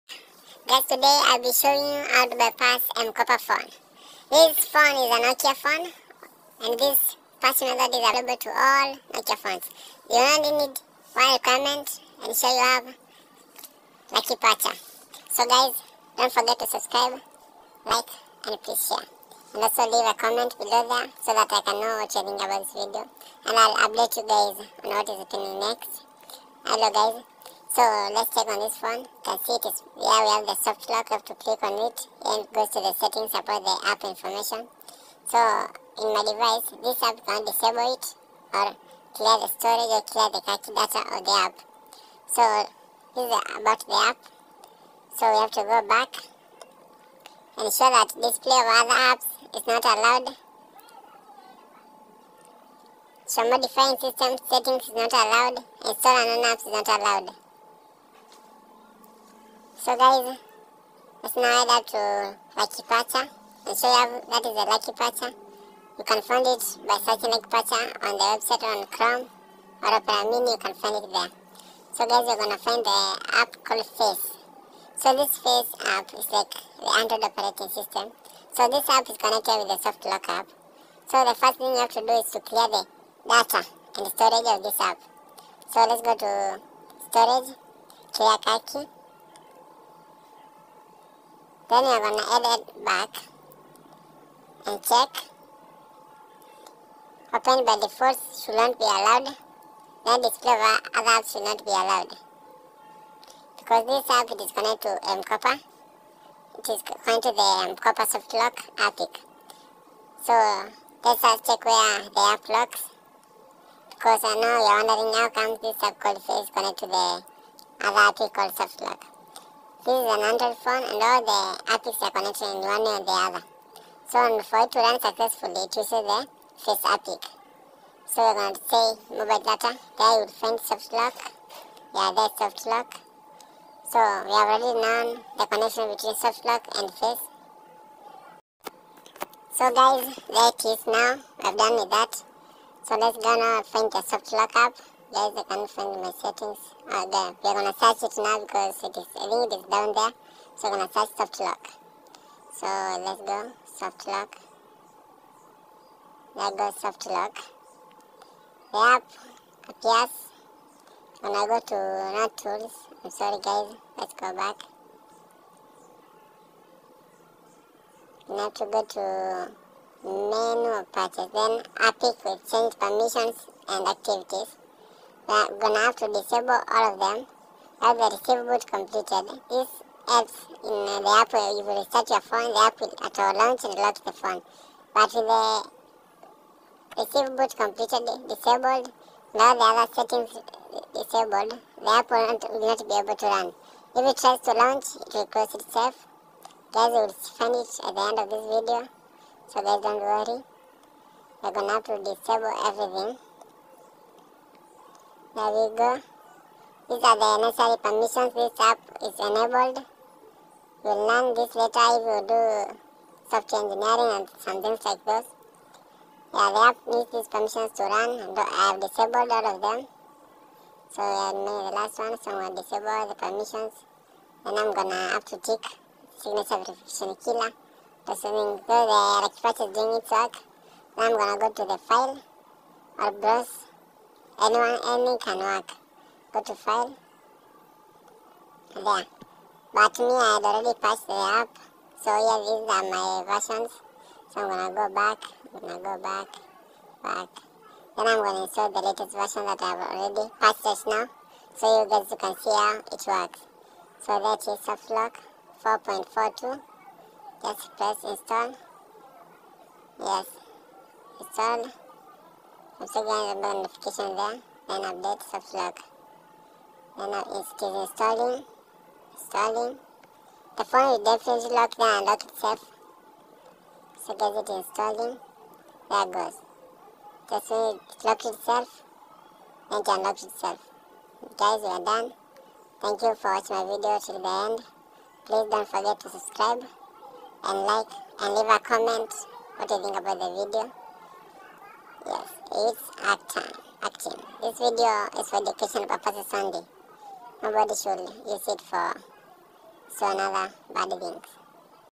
permanent unlock nokia c32,c22 mkopa phones using lucky patcher
permanent unlock nokia c22,c32 mkopa phones, this video is a tutorial showing step by step bypassing all nokia mkopa phones, I recommend this video to mkopa users to be beneficial